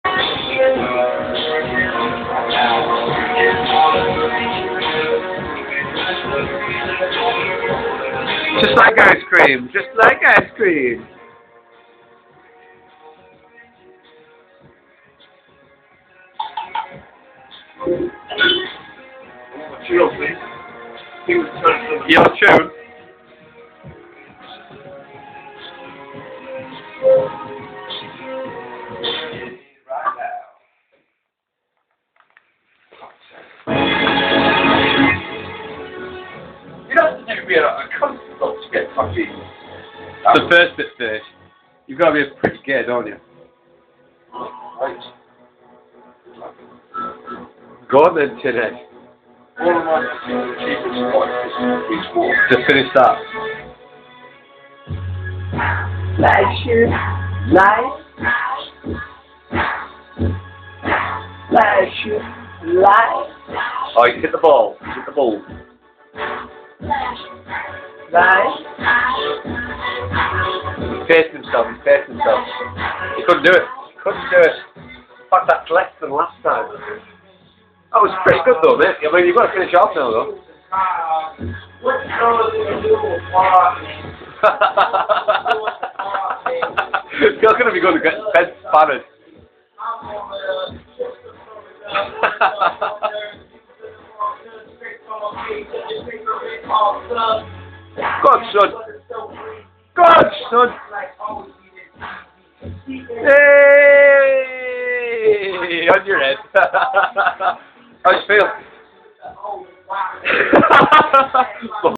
Yard of Lager 2
Tin ead eats lager like ice cream then attempts the neckage! Can he do it?